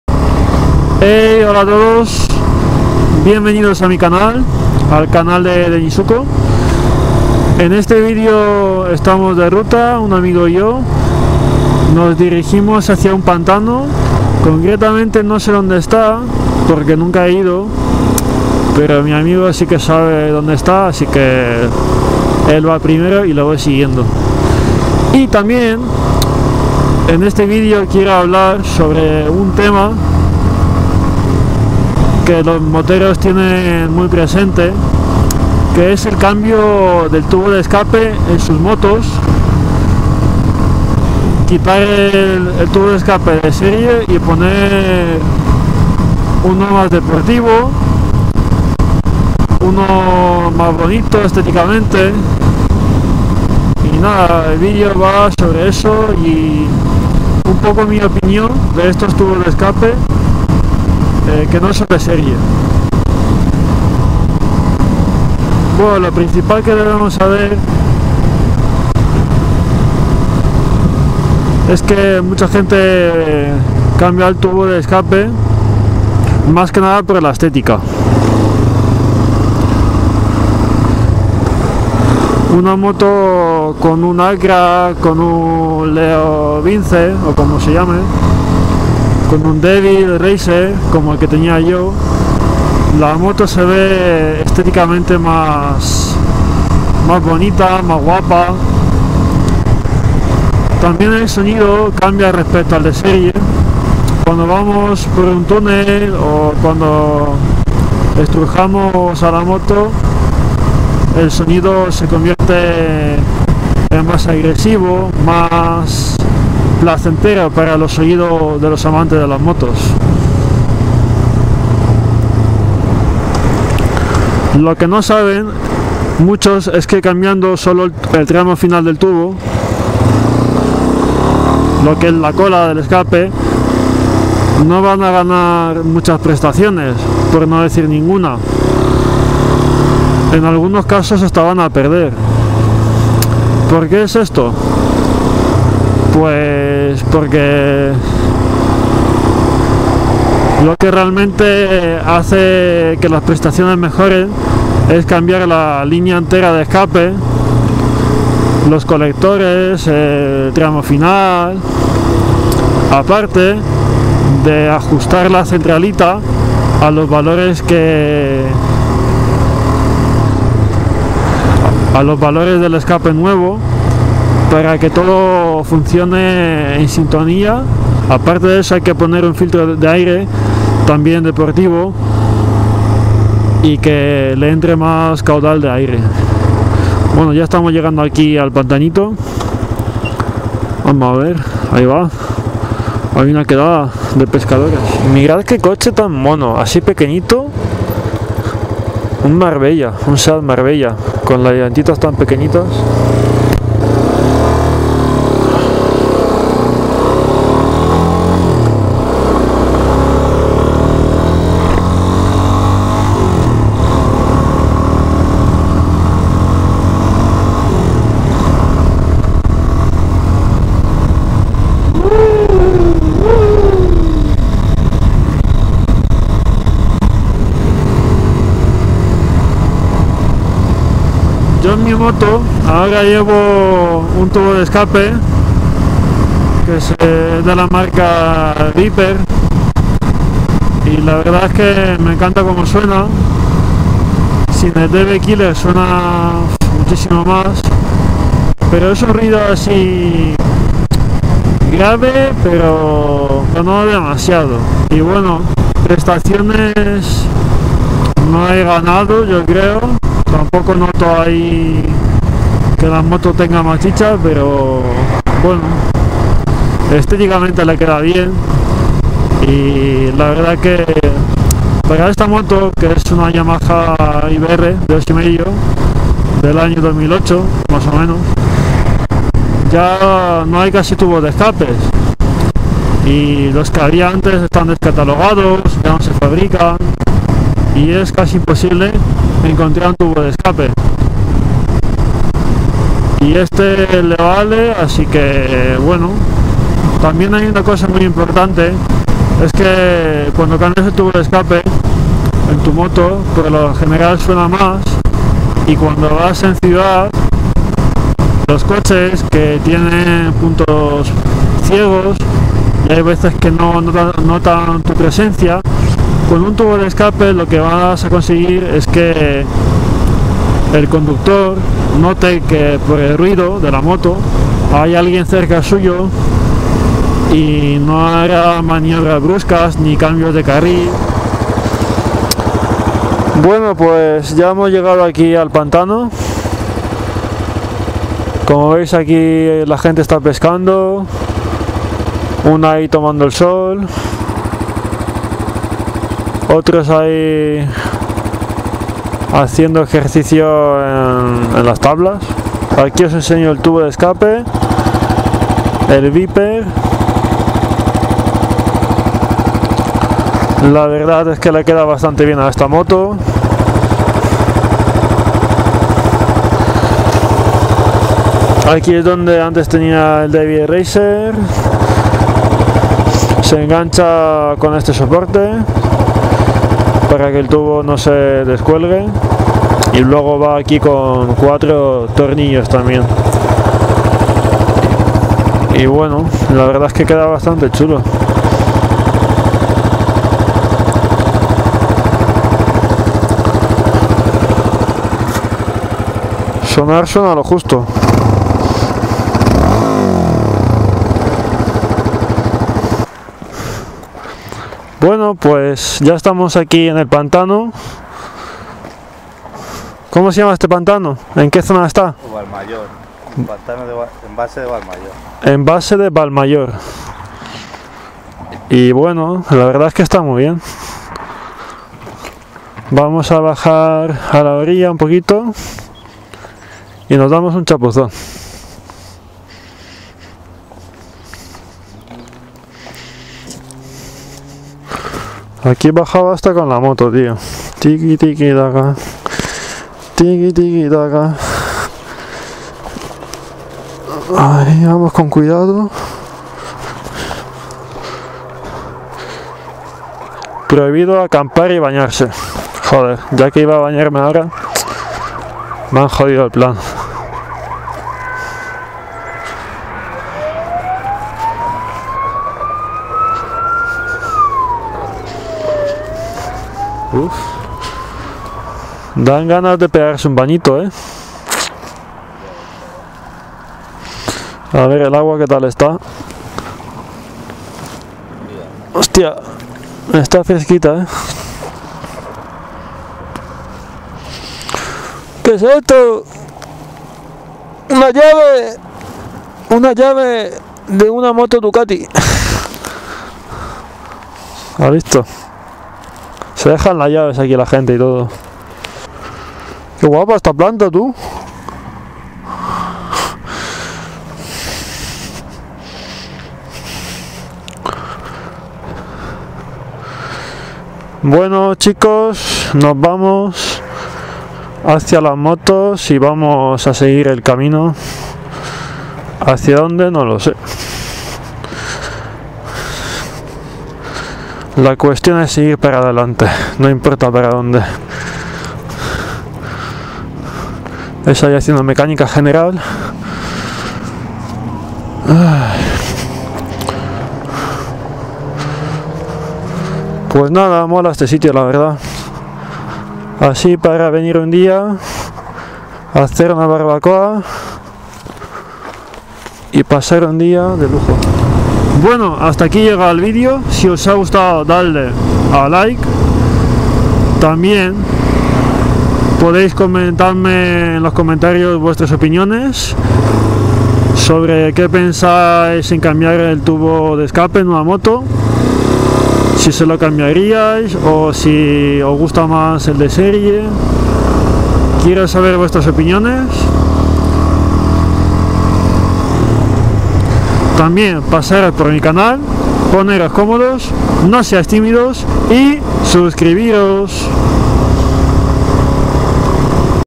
Todo Lo Que Debes Saber Al Cambiar Tubo Escape De Moto | Motovlog
Espero que os guste esta ruta y os enseño el nuevo escape que lleva mi moto.

Compártelos con tus amigos y déjame tus comentarios para que sepa qué opinas y sobre qué temas quieres que hable.